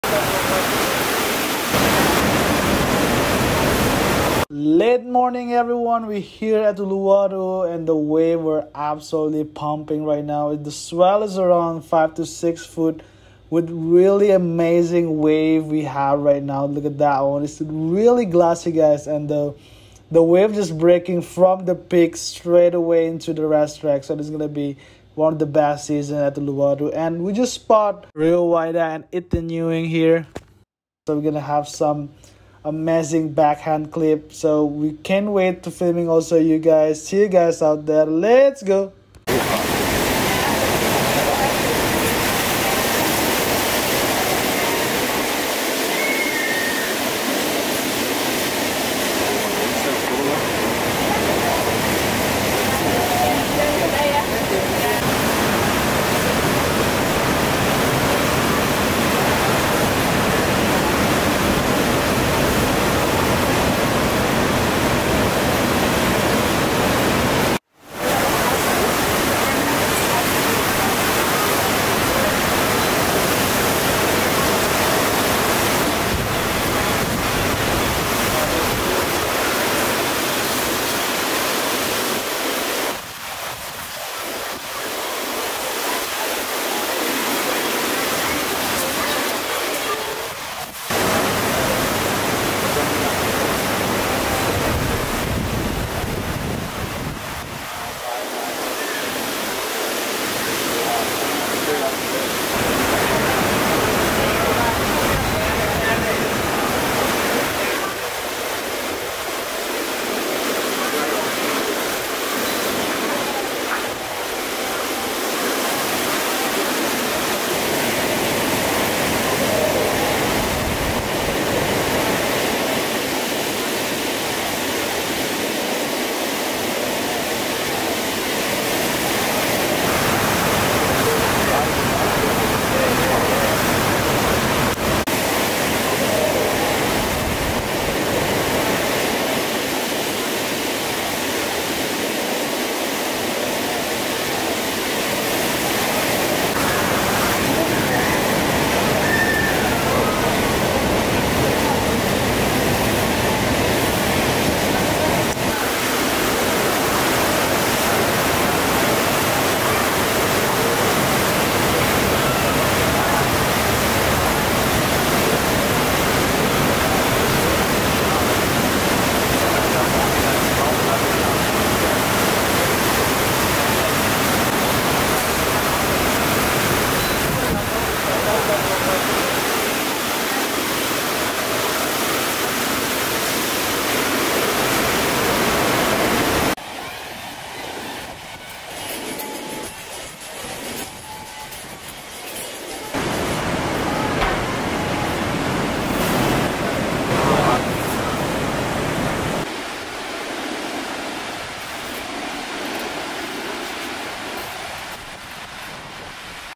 Ethan Ewing & Rio Waida Outstanding Backhand at Uluwatu
Rio Waida & Ethan Ewing have put an outstanding backhand performance at Uluwatu today. Let's goo!!

Location
Uluwatu, Bali

Shooting Month
October 2024

Thanks for all the surfers in this video, without you guys we couldn't do what we love!